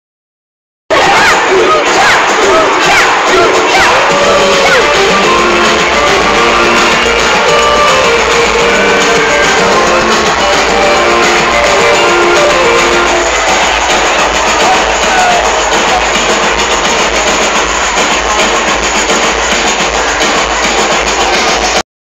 Lucha dragons...crowd cheering for the luchas
Shaun now 7 a massive wrestling fan, we went to see WWE live at the echo arena in Liverpool 8/11/2015